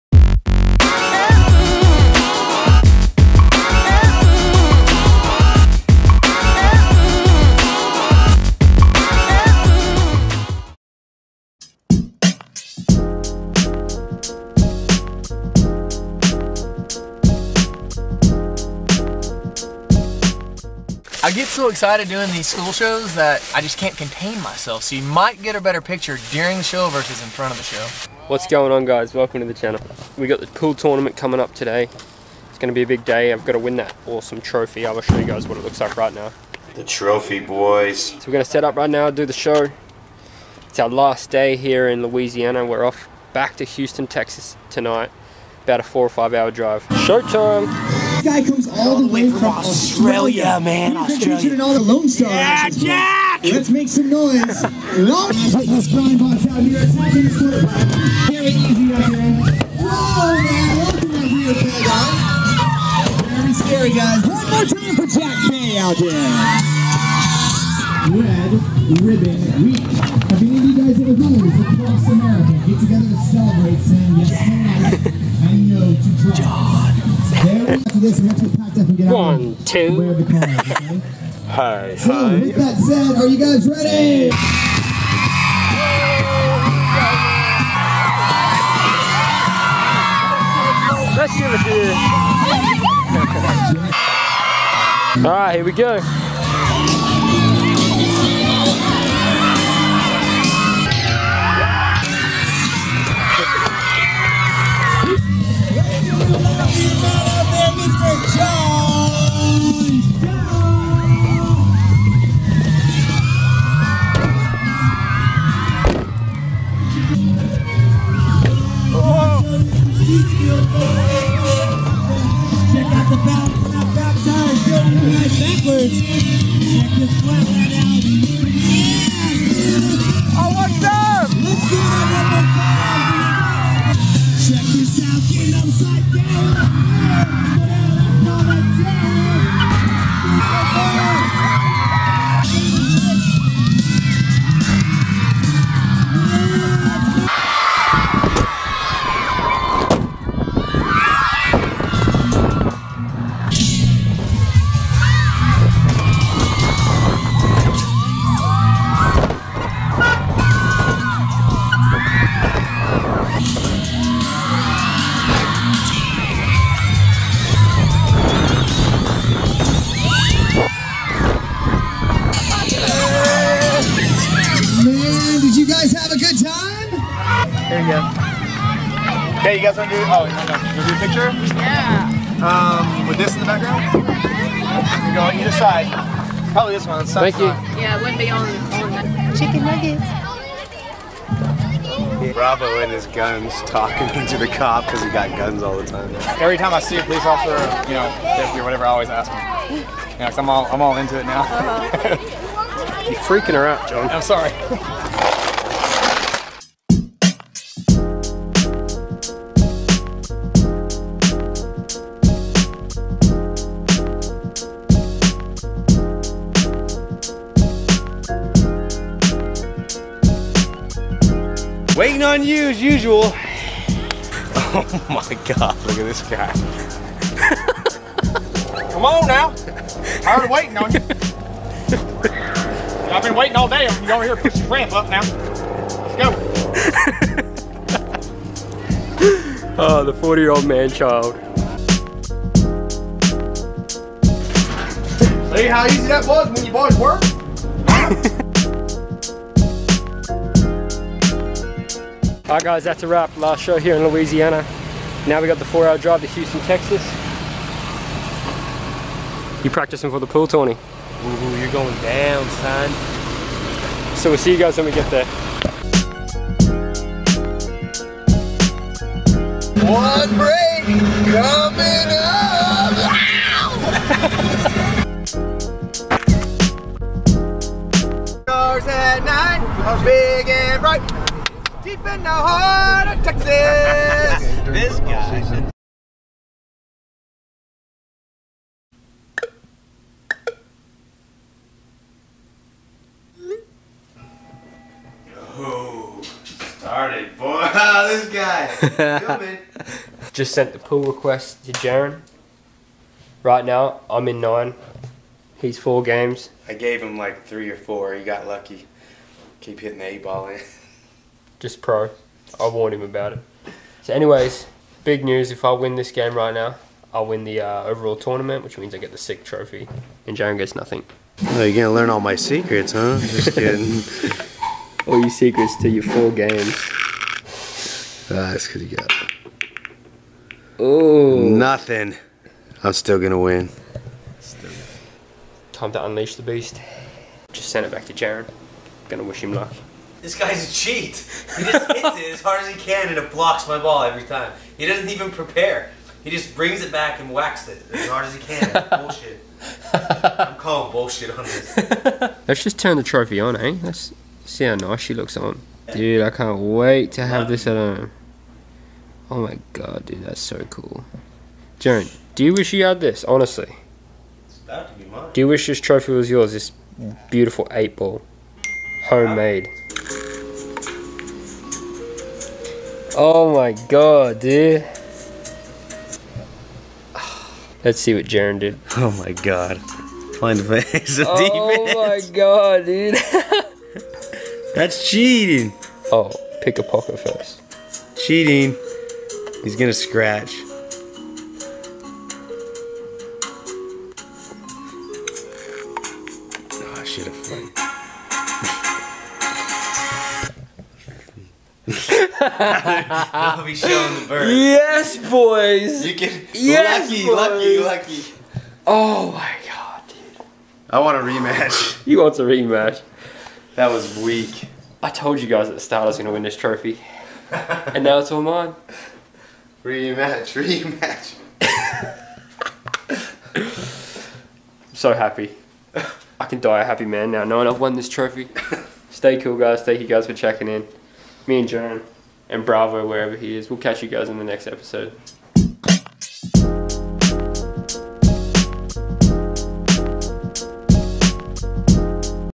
I JUST GOT CROWNED WORLD CHAMPION !!!
Hey whats up guys :) !!
In this Video a pocket pool world champion is crowned !! We started the day off in Louisiana for our last day of show there before heading back to Huston Texas ! I filmed some onboard clips so you guys can see what it looks like from my perspective!!

Social media -

jackbmx1